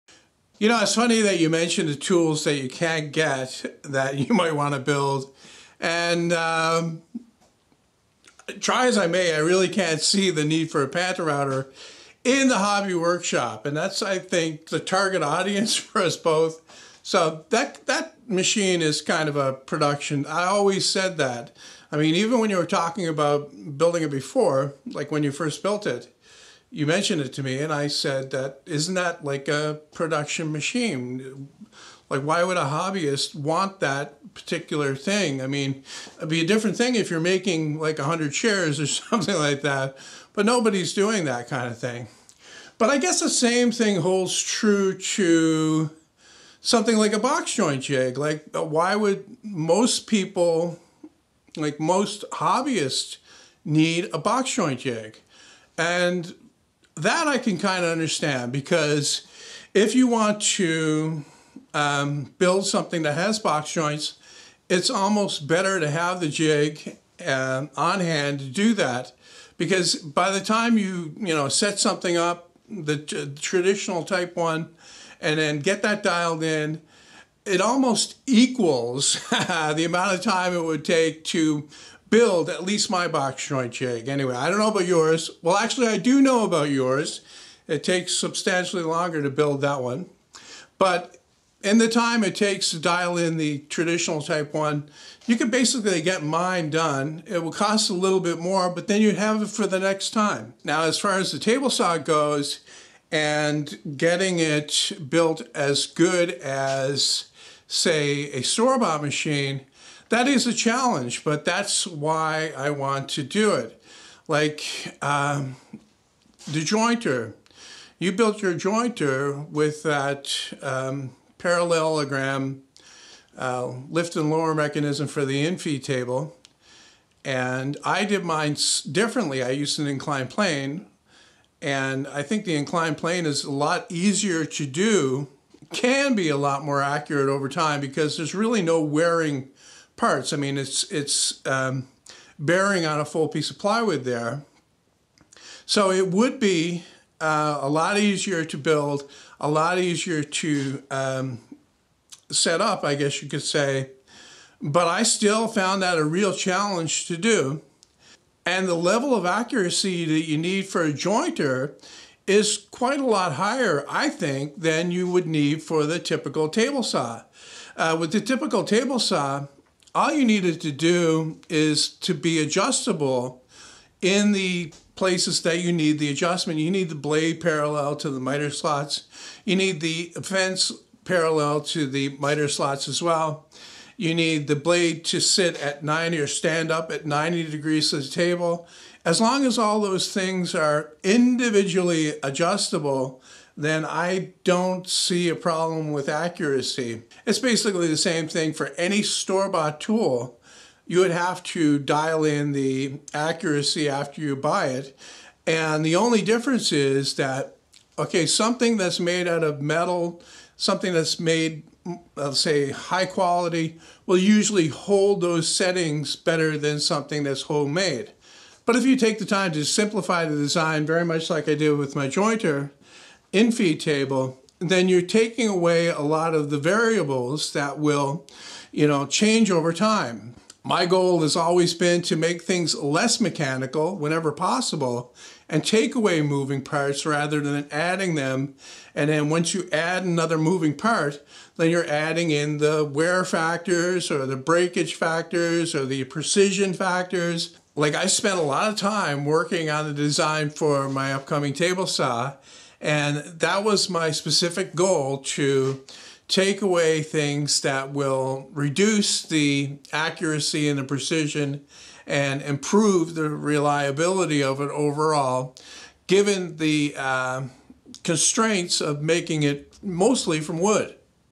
How accurate and dependable are homemade tools?
I think it is possible to make a table saw mainly from wooden components that is as dependable and accurate as a factory made machine. However, like I said in previous videos, accuracy and precision has more to do with the operator and his skill and ability.